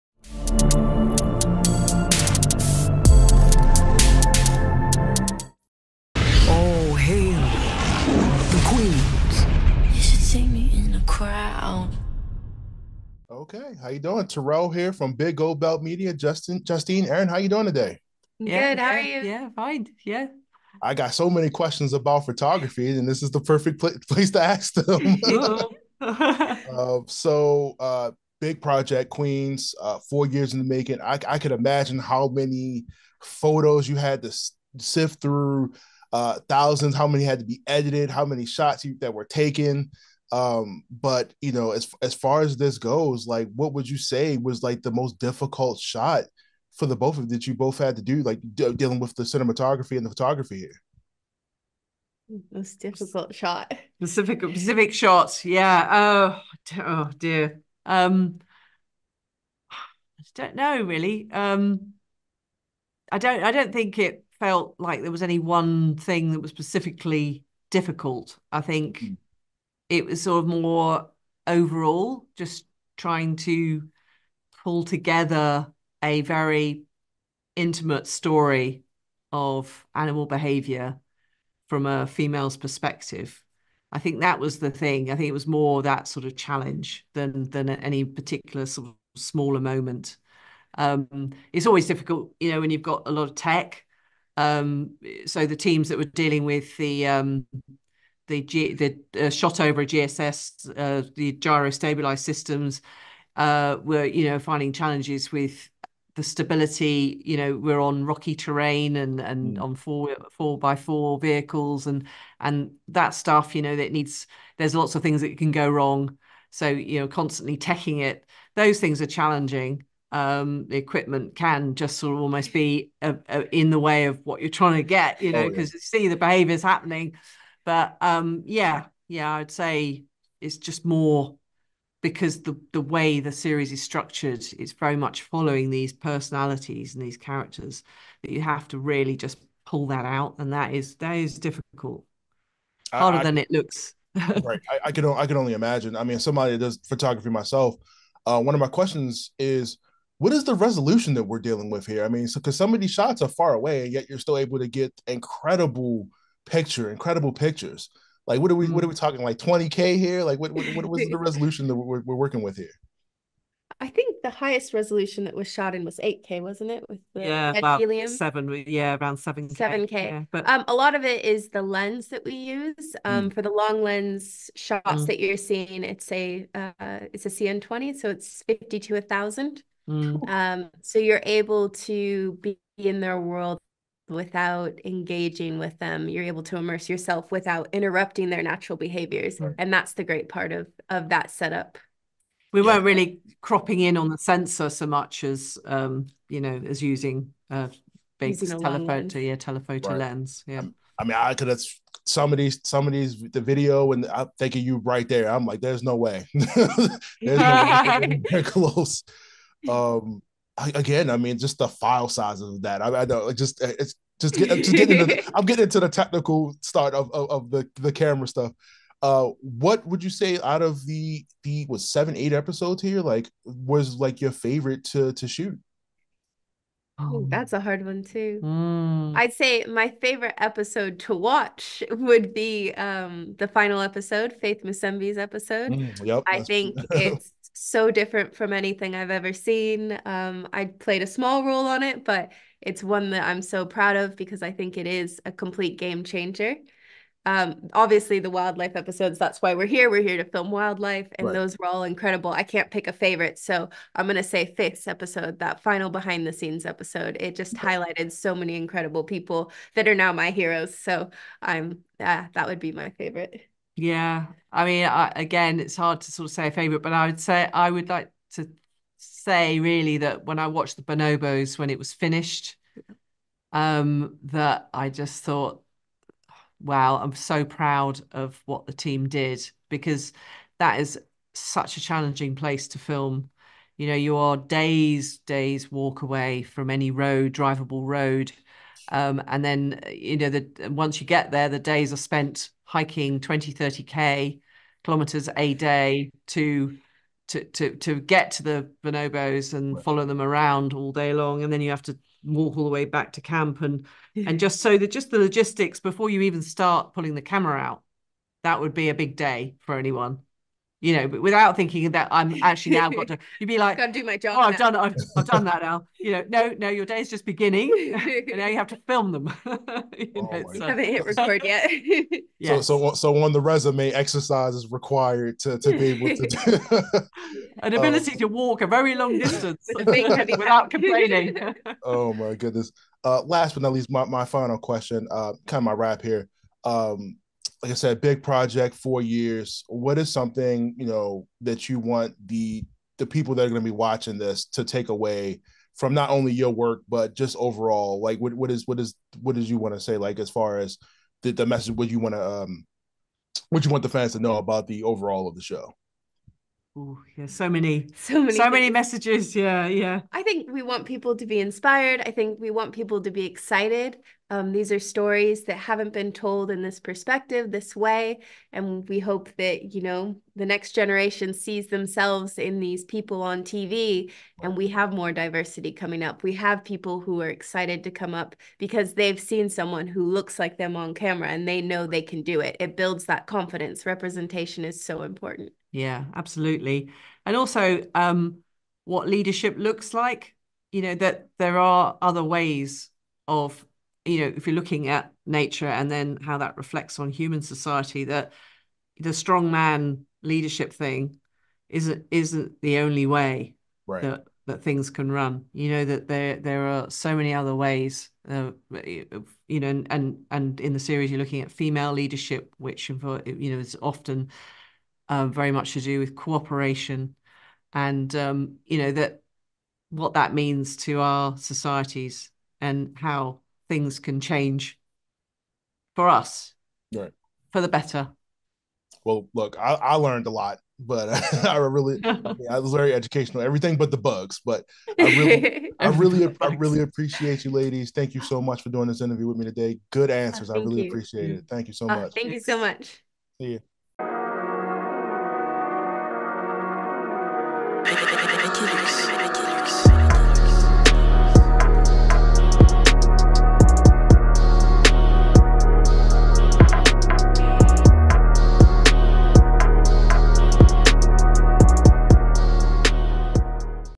Justine Evans and Erin Ranney Interview | National Geographic "Queens"
Justine Evans and Erin Ranney Interview by Big Gold Belt Media with @AroundTheGeek

Justine Evans - Director of Photography
Erin Ranney - Cinematographer
Synopsis:
 The wildest places on the planet have always been home to powerful leaders, but this is a story of a new hero – fierce, smart, resilient and ... female. QUEENS features matriarchies and female leaders around the world to tell a story of sacrifice and resilience but also of friendship and love. These QUEENS aren’t always kind or gentle, letting nothing come between them and the success and safety of their families. Guided by award-winning actress Angela Bassett’s powerful narration, QUEENS brings the natural world into focus through the female lens for the very first time. Four years in the making and helmed by a female-led production team from around the world – groundbreaking in the natural history space – the seven-part series leverages cutting-edge technology to reveal surprising insights into how females in the natural world rise to power, often relying on cooperation and wisdom over brute strength to get ahead. The final episode of the series celebrates the women who have gone to the ends of the Earth and dedicated their lives to documenting and protecting animal queens. We don’t call her Mother Nature for nothing. All hail ... the QUEENS.

QUEENS is produced by Wildstar Films for National Geographic. For Wildstar Films, Vanessa Berlowitz is executive producer. Janet Han Vissering is senior vice president of development and production, National Geographic.